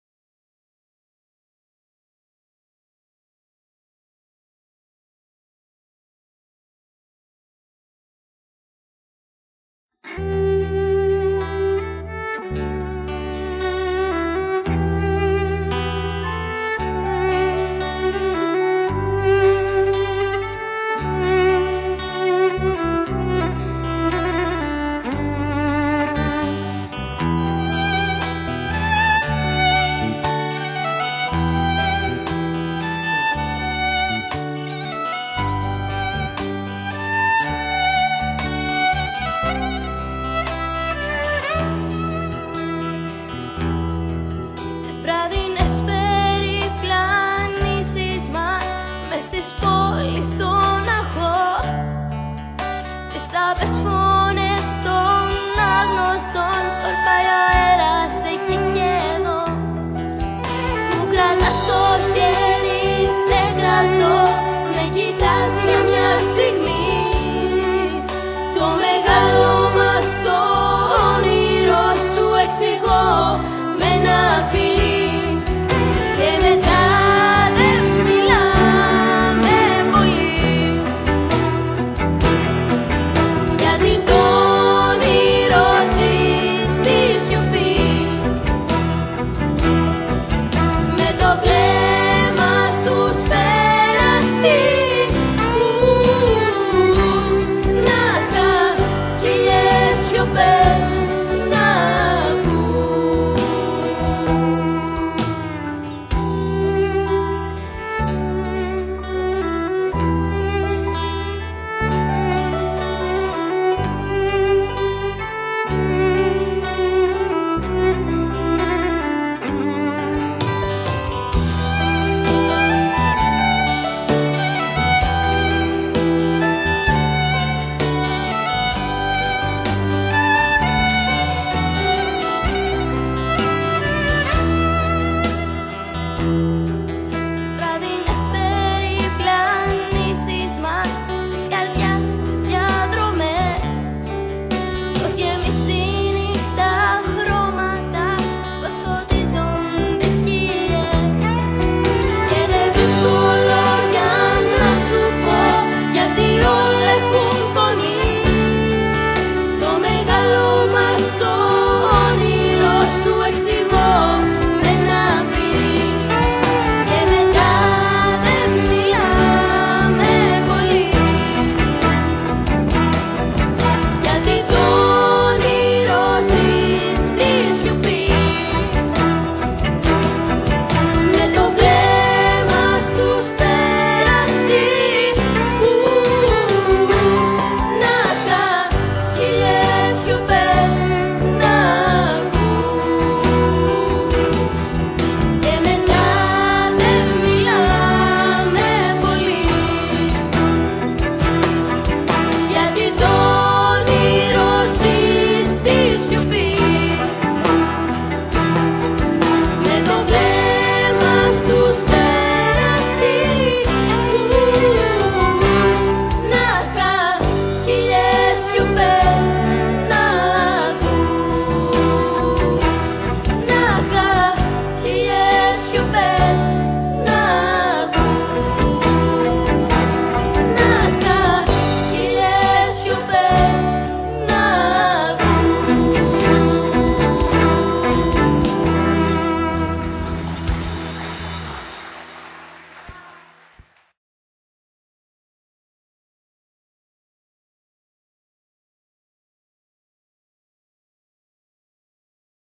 Μουσικό Εργαστήρι "Χιλιες σιωπες" LIVE 2013
Μουσικό Εργαστήρι Γυμνασίου Βάμου
"Χίλιες Σιωπές"
Θέατρο "Δ. Βλήσίδης"  6 Απρ. 2013
Ολυμπία Πολίτη
Νικολέτα Χατζή
Νεάντα Μπραχουσάι
Κιθάρα: Δημήτρης Καβαλινάκης
Ντραμς: Γιώργος Νιαυραδάκης